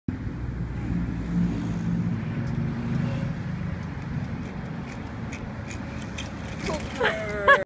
Spooky the Boston Terrier learning how to skateboard- Lesson 2
Spooky the Boston Terrier is learning how to skateboard.  This is her second time on the skateboard and she's getting more comfortable riding on it.